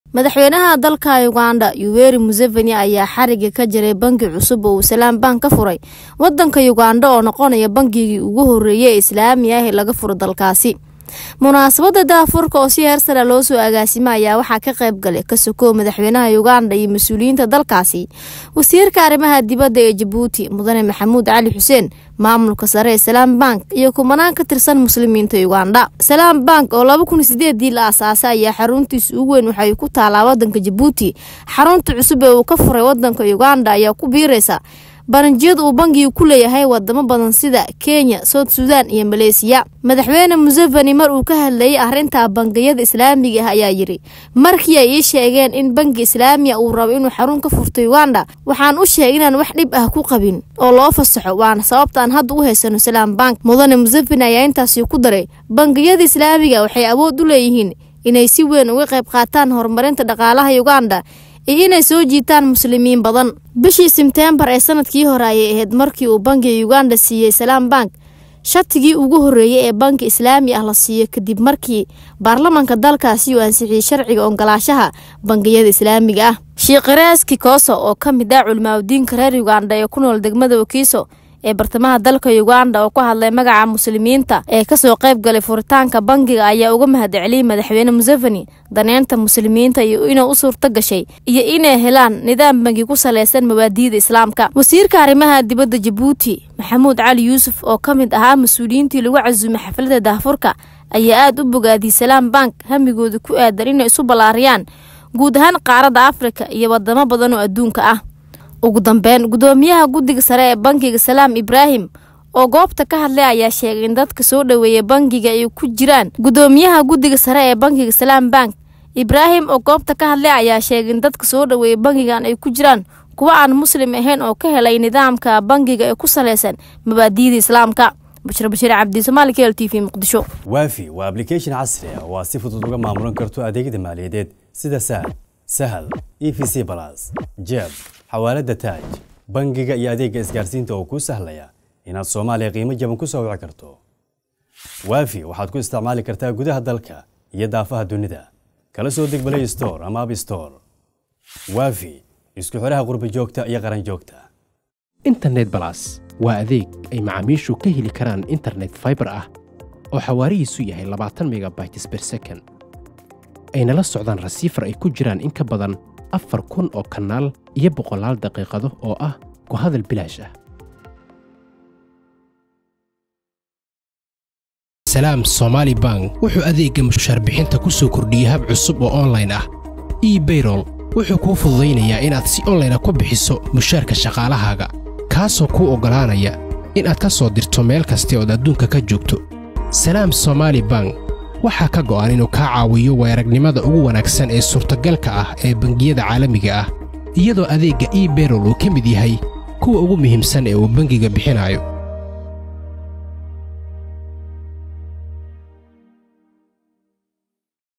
Madaxweynaha Dalka Uganda, ayaa xariga ka jaray banki cusub oo uu Salaam Bank
Madaxweynaha Dalka Uganda, ayaa  xariga ka jaray banki cusub oo uu Salaam Bank ka furey wadanka Uganda oo noqonaya bangigii ugu horeeyey ee Islami ah ee laga furo dalkaas.